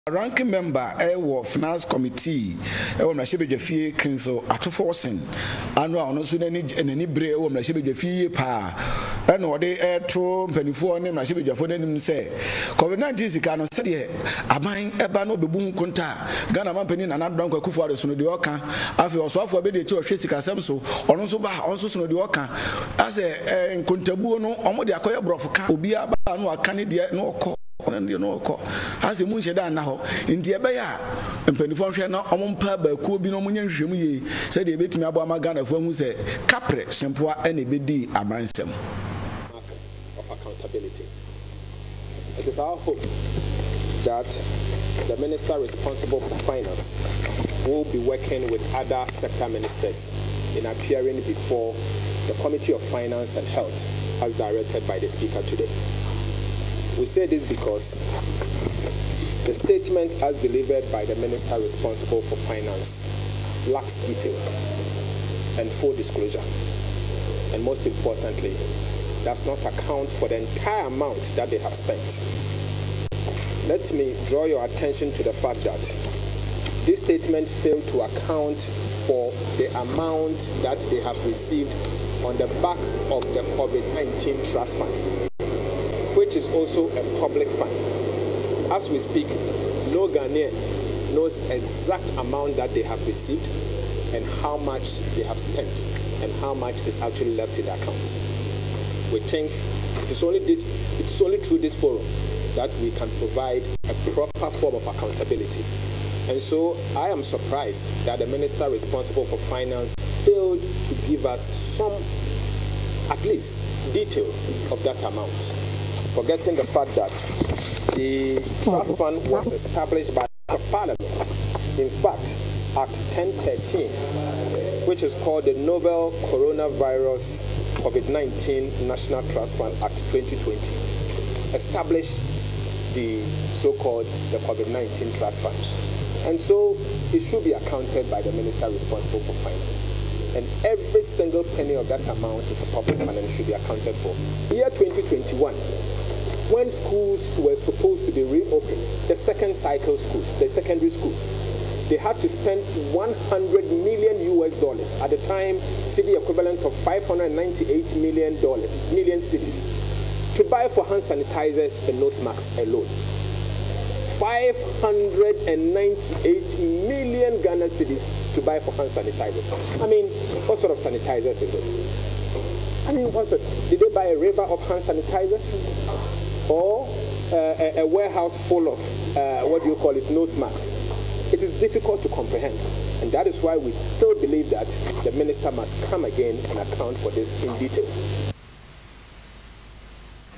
COVID - 19 EXPENDITURE - PROBE INTO $300M SPENT - ATO FORSON
A Ranking Member for the finance committee, Cassiel Ato-Forson in an interview at Parliament said the Finance Minister was supposed to give an account in the Covid-19 expenditure but rather gave a statement and he thinks the minister lacks details and full disclosure with regards to the expenditure. He again revealed that the minister in his statement said an amount of $100m was used to purchase nose masks, sanitizers and veronica buckets for secondary schools to curb the spread of the Covid-19. He then called on the Speaker of Parliament to set a Committee to probe in that Expenditure.